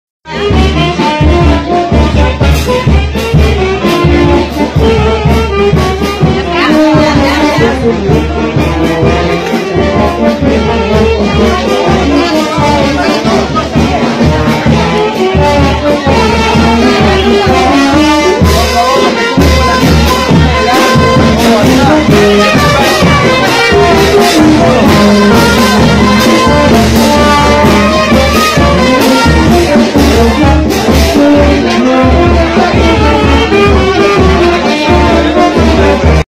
Verónica Escobal, realizo Caminata en Laredo
Verónica Escobal candidata a la alcaldía de Trujillo realizo una caminata en el distrito de Laredo acompañada de Geanmarco Quesada y Luis Jose Gonzales,